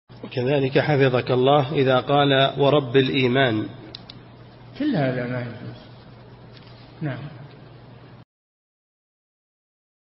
[200 -939] ما حكم الحلف بلفظ "ورب الإيمان"؟ - الشيخ صالح الفوزان
-[200 -939] السؤال: وكذلك حفظك الله إذا قال ورب الإيمان ؟
الجواب: كل هذا ما يجوز.
للاشتراك في قناة الشيخ صالح الفوزان

للاستفسار والإقتراحات والدعم
رئيس الفريق
لمشروع كبار العلماء